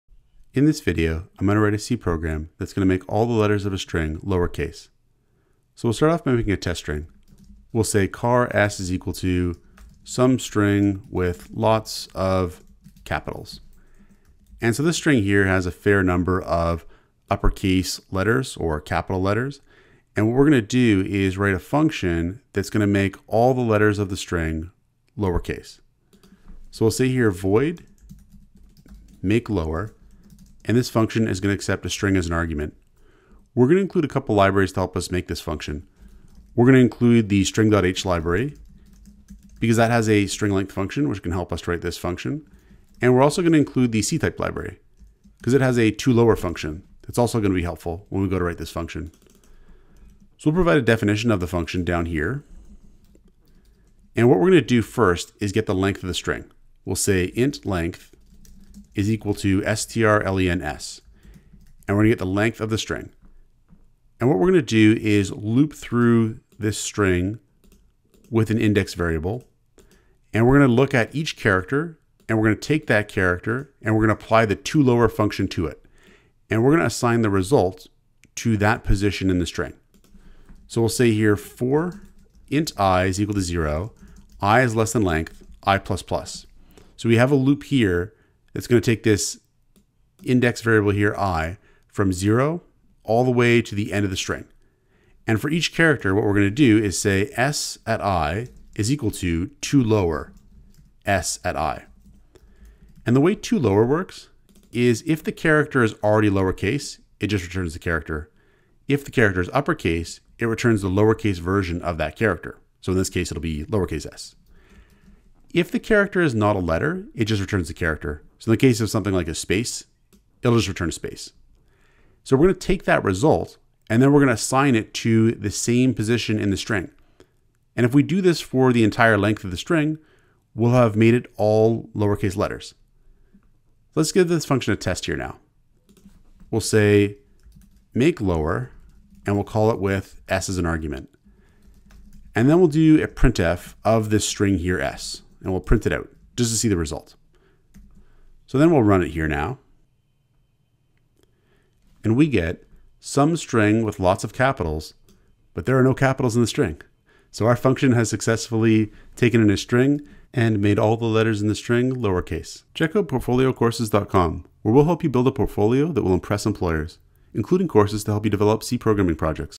Make All String Letters Lowercase | C Programming Example
Make all letters of a string lowercase in C. Source code to build a portfolio that will impress employers!